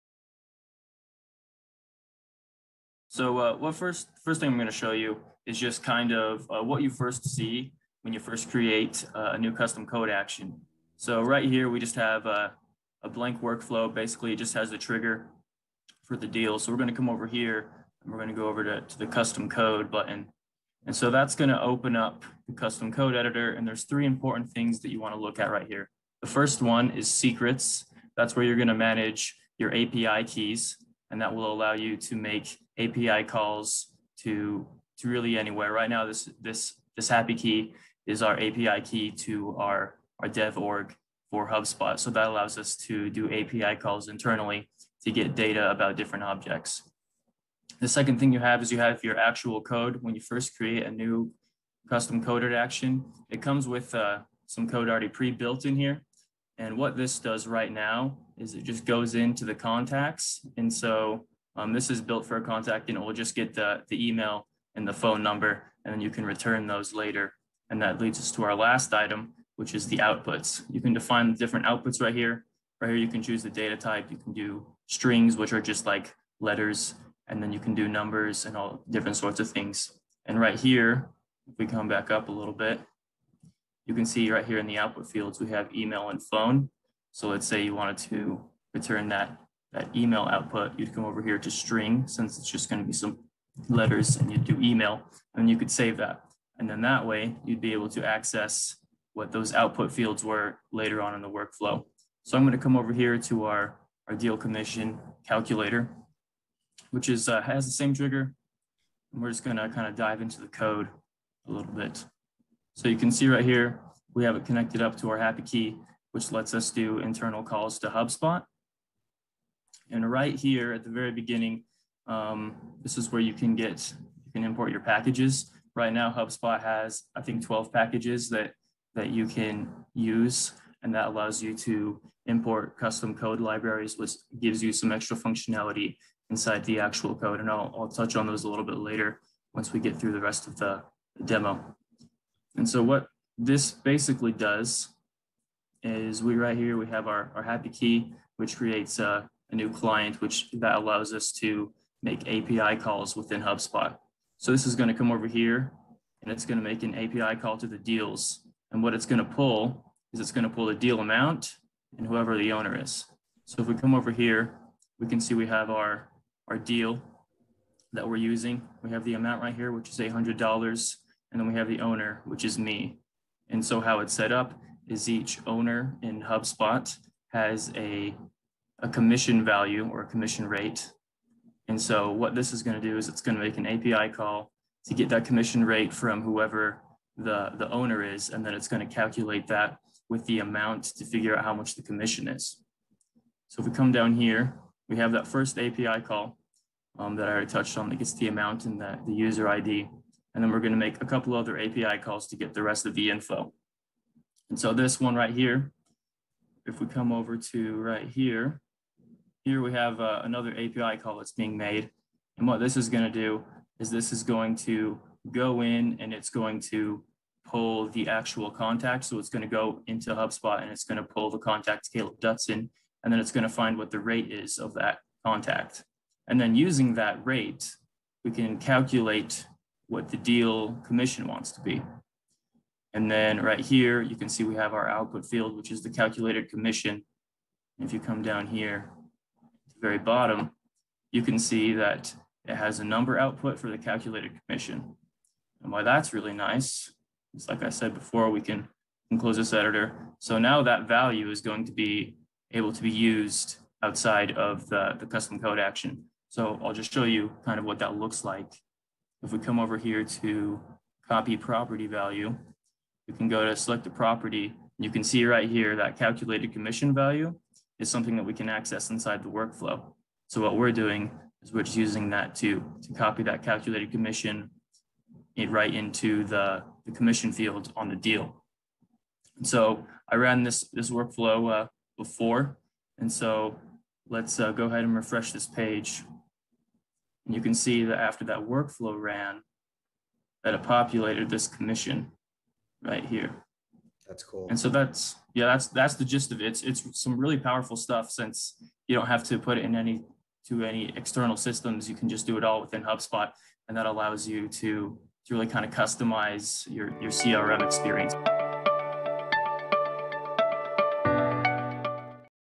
HubSpot Programmable Automation Demo
Demo of how to use HubSpot's new feature, programmable automation. Clip taken from May 2021 webinar Operations Hub: Programmable Automation.